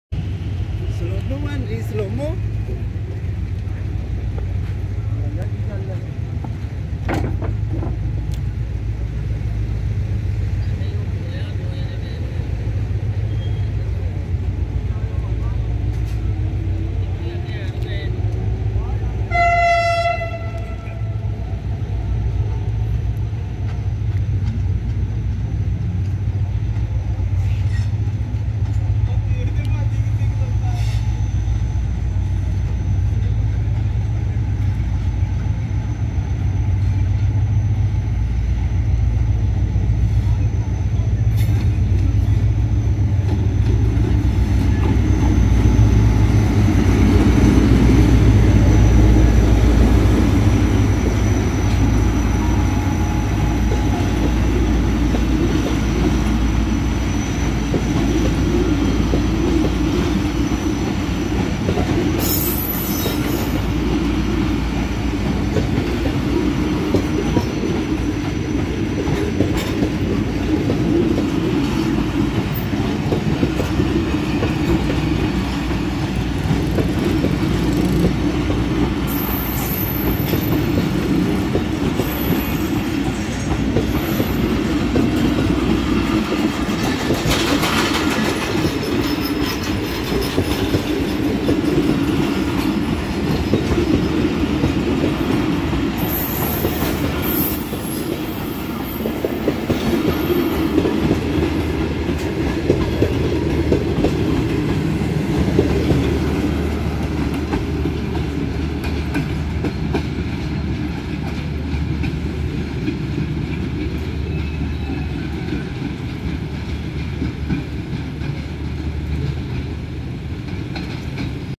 27UP Shalimar Express is Departing From Toba Tek Singh With HGMU-30R-8222 | MirXa Haris
Sit Back, Relax & Enjoy The Video & Please Give Me Your Feedback.

Watch The Video Till End & You Will Like This Video